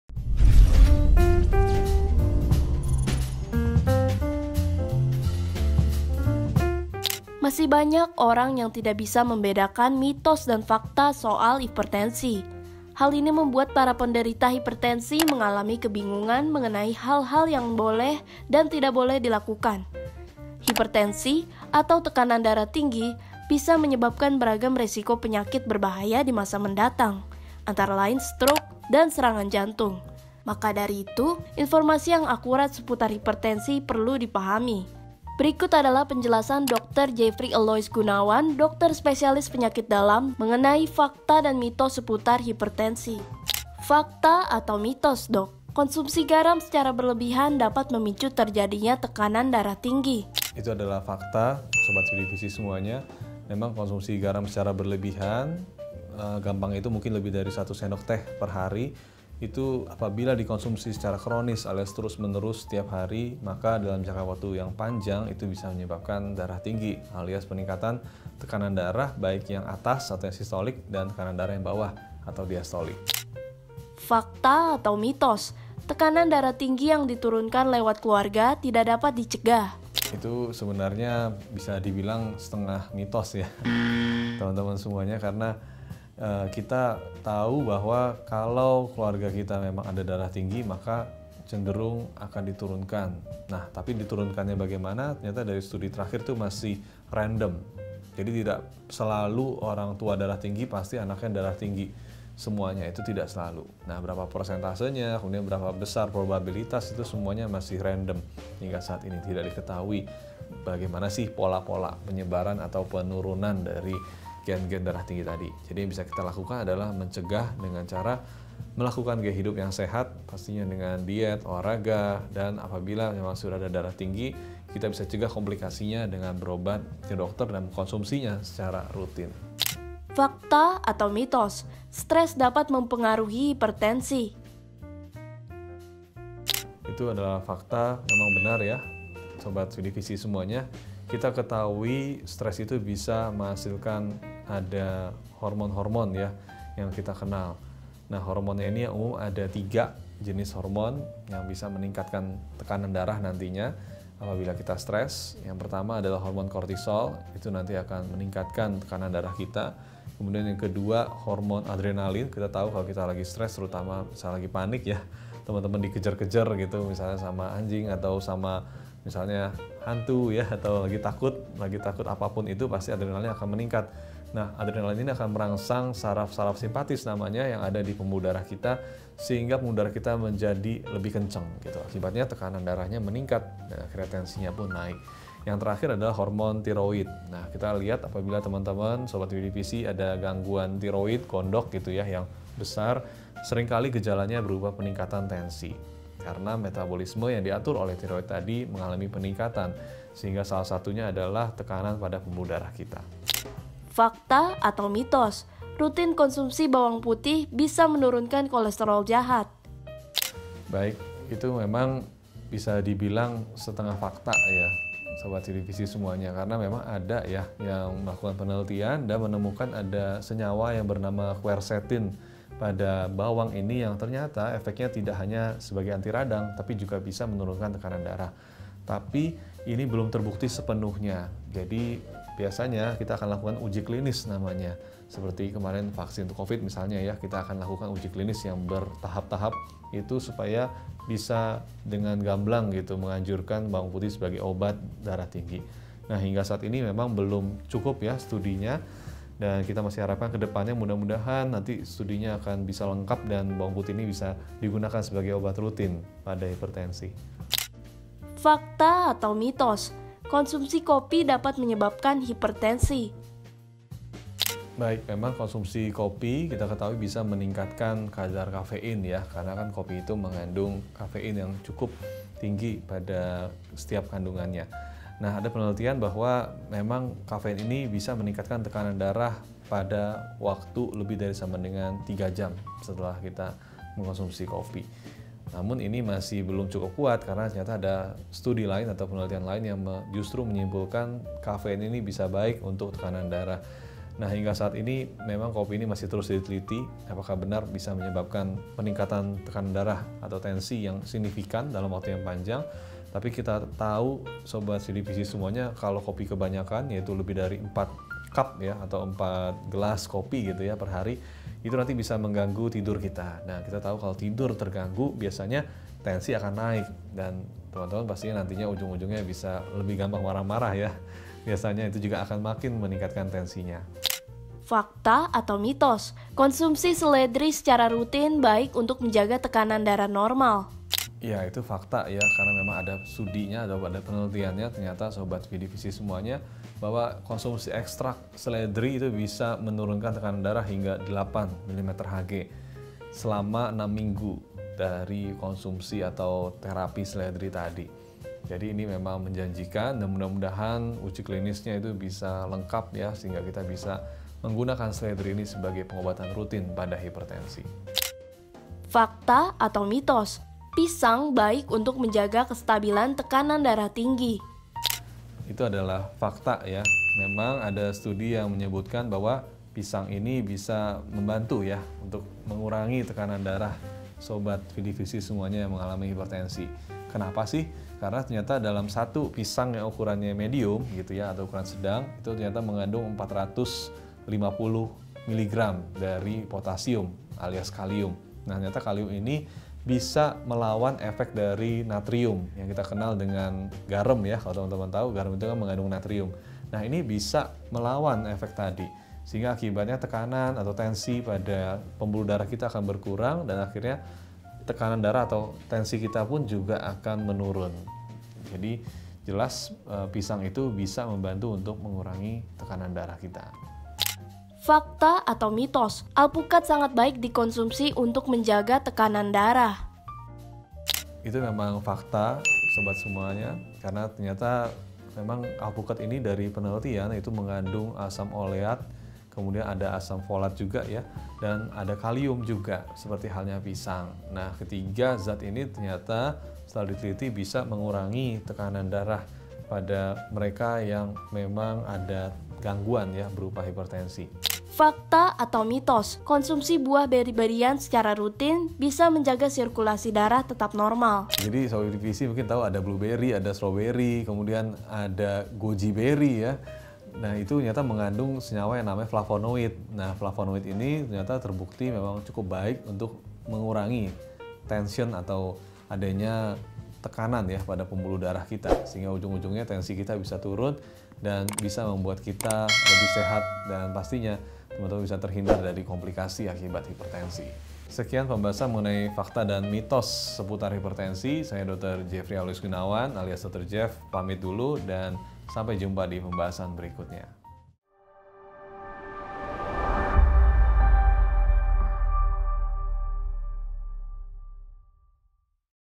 Idap Hipertensi? Ini Makanan Yang Baik Untuk Turunkan Tekanan Darah Tinggi | Kata Dokter #178
kata dokter - Masih banyak orang yang tidak bisa membedakan mitos dan fakta soal hipertensi. Hal ini membuat para penderita hipertensi mengalami kebingungan mengenai hal-hal yang boleh dan tidak boleh dilakukan. Hipertensi atau tekanan darah tinggi bisa menyebabkan beragam risiko penyakit berbahaya di masa mendatang, antara lain stroke dan serangan jantung. Maka itu, informasi yang akurat seputar hipertensi perlu dipahami. Berikut adalah penjelasan dr. Jeffri Aloys Gunawan, Sp.PD, CHt, FINASIM, dokter spesialis penyakit dalam mengenai fakta dan mitos seputar hipertensi.